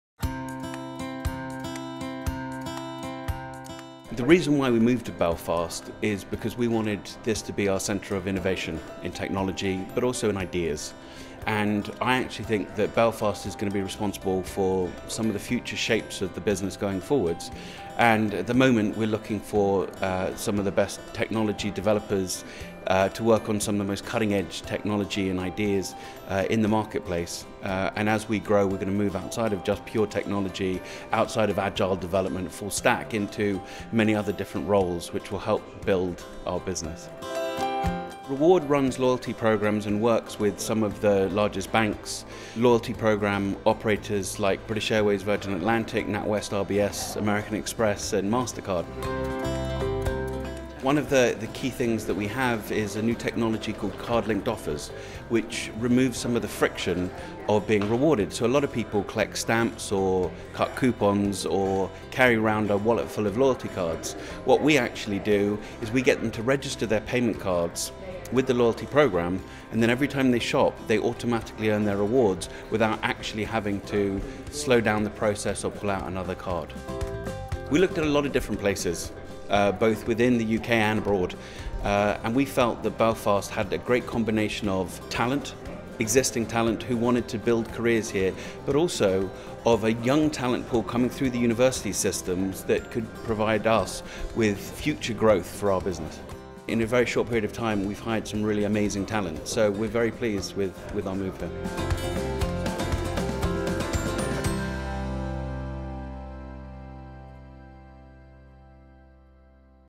Gavin Dein | Reward
Gavin Dein, Founder & CEO of Reward, Europe’s largest Card Linked Offers company chooses Belfast for its engineering hub for innovation. He discusses the benefits for his company locating in Belfast.